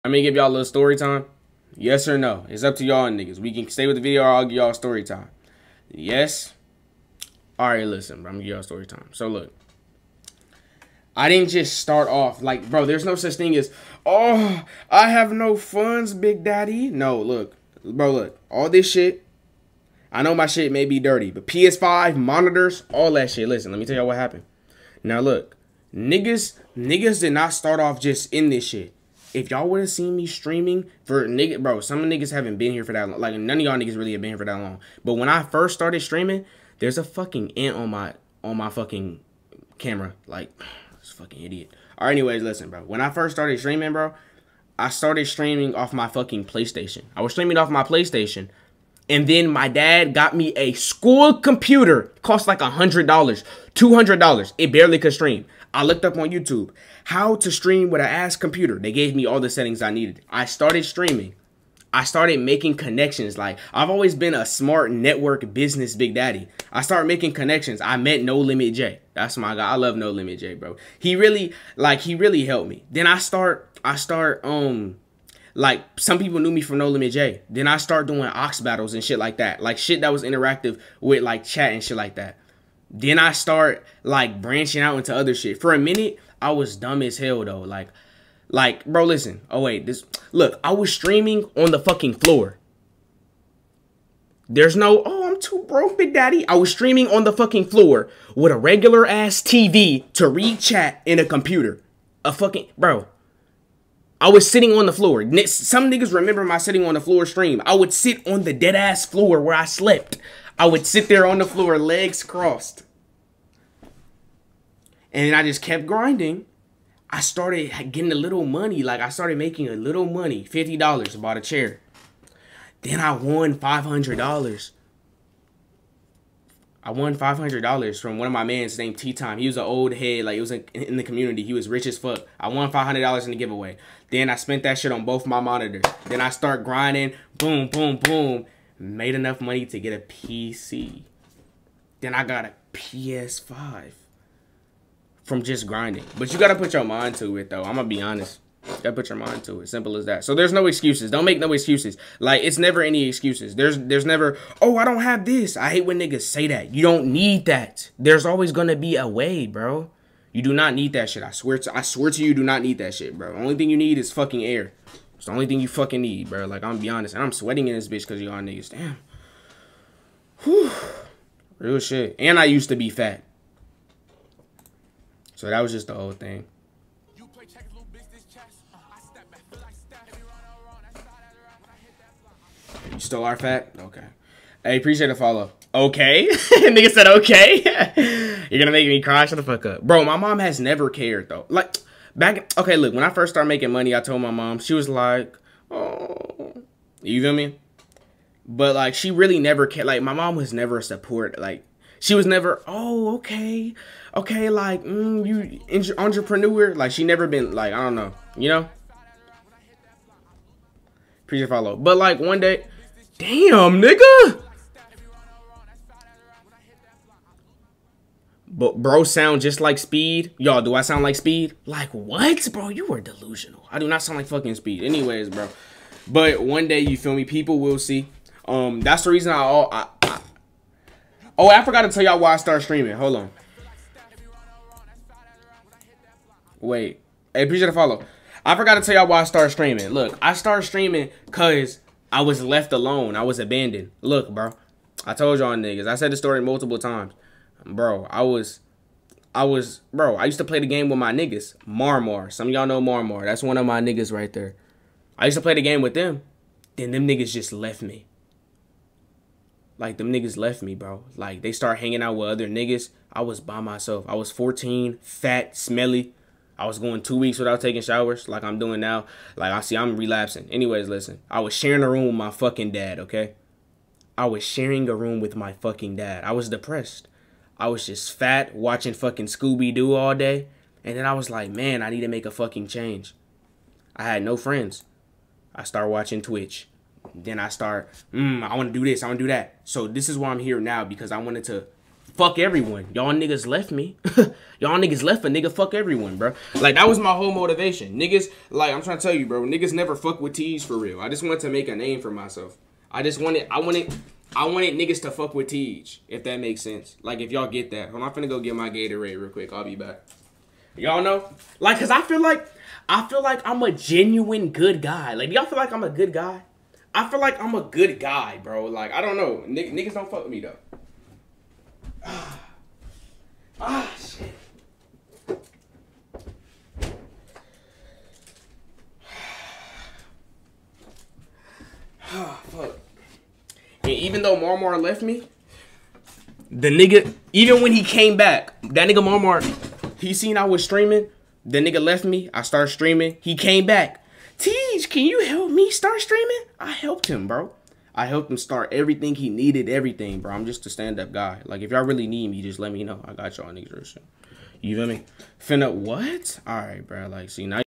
Teige Explains Why He Became A Streamer..
Teige Explains How He Started Streaming…

NO HATE, NO BODY HURT. I FOLLOW COMMUNITY GUIDELINES
THANK YOUi